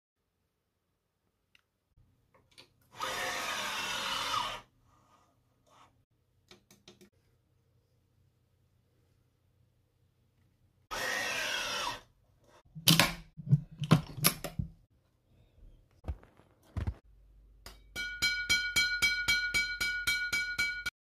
Yonex Exbolt 65 33x35 lbs + 10% pre-stretch
Stringing the new Yonex Exbolt 65 with 33x35 lbs + 10% pre-stretch.
Sounds pretty good to me and looking forward to try it out and see how it feels!